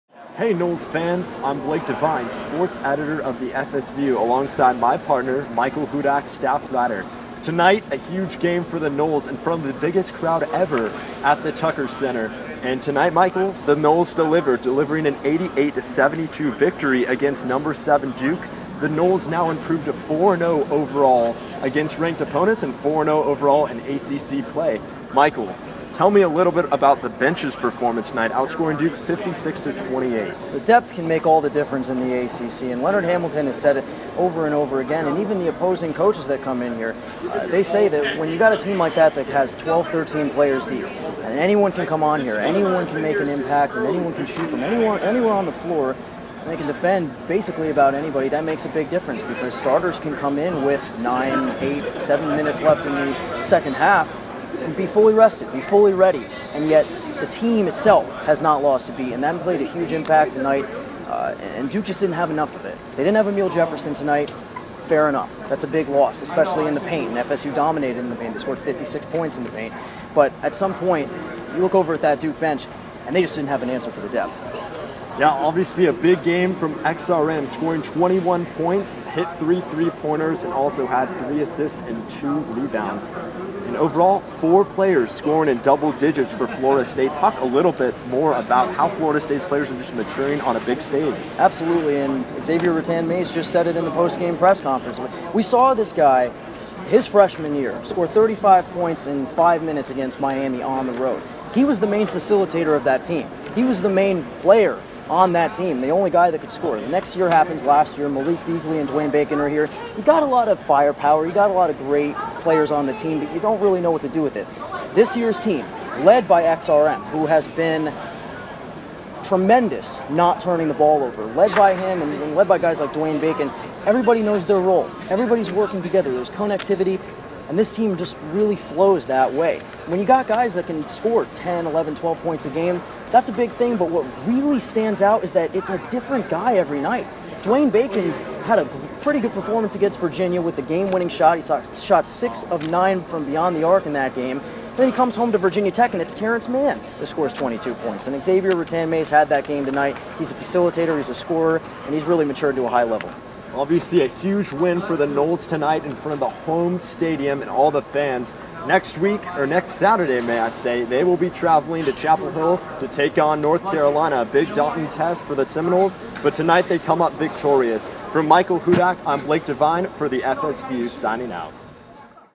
FSU vs Duke Basketball Recap
FSView's Sports Editor Blake Devine and Staff Writer Michael Hudak break down FSU basketball's 88-72 victory over Duke.